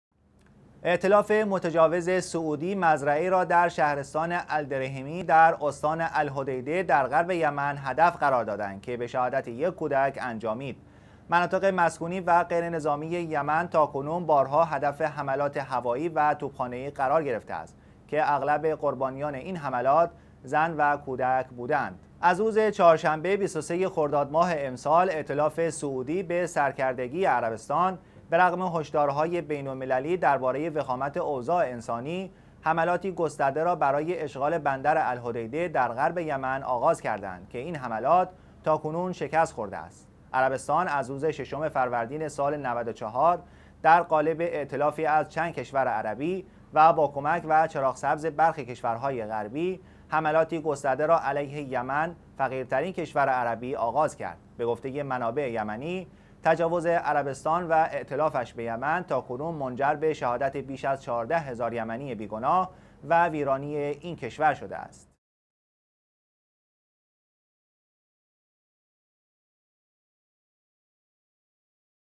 شهادت یک کودک یمنی در مزرعه ای در استان الحدیده
یک کودک بر اثر حمله نیروهای ائتلاف_سعودی به سرکردگی عربستان در غرب یمن به شهادت رسید.
ائتلاف سعودی مزرعه ای را در شهرستان الدریهمی در استان الحدیده در غرب یمن هدف قرار داد که به شهادت یک کودک انجامید.
مناطق مسکونی و غیرنظامی یمن تاکنون بارها هدف حملات هوایی و توپخانه ای قرار گرفته است که اغلب قربانیان این حملات زن و کودک بوده اند.
ائتلاف سعودی به سرکردگی عربستان از روز چهارشنبه 23 خرداد ماه امسال به رغم هشدارهای بین المللی درباره وخامت اوضاع انسانی، حملات گسترده ای را برای اشغال بندر الحدیده در غرب یمن آغاز کرده اند که این حملات تاکنون شکست خورده است.
عربستان از روز ششم فروردین سال 94 در قالب ائتلافی از چند کشور عربی و با کمک و چراغ سبز برخی کشورهای غربی، حملات گسترده ای را علیه یمن، فقیرترین کشور عربی آغاز کرد.
به گفته منابع یمنی، تجاوز عربستان و اائتلافش به یمن تاکنون منجر به شهادت بیش از 14 هزار یمنی بی گناه و ویرانی این کشور شده است.

چهار شنبه - ۱۷ مرداد ۱۳۹۷